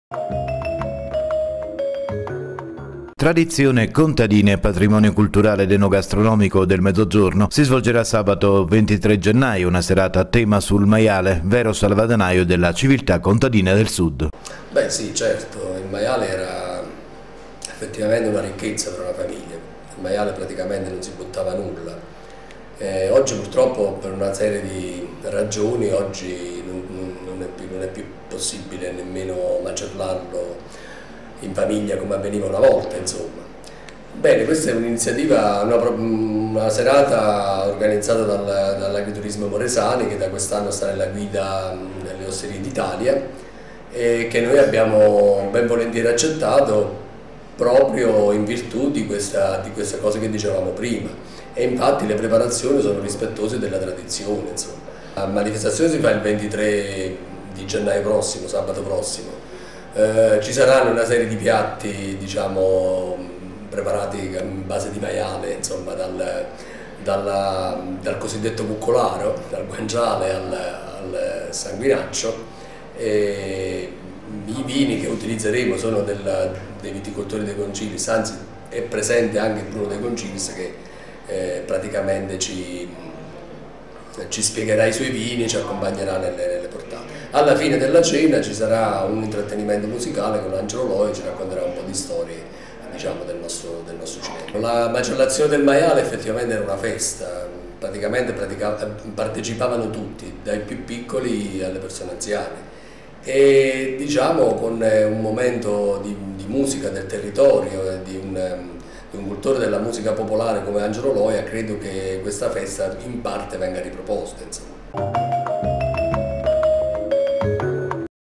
Slow Food Condotta Gelbison Serata a tema sul maiale.mp4
Serata a tema sulla tradizione contadina e sull'animale per eccellenza di questa tradizione: il maiale.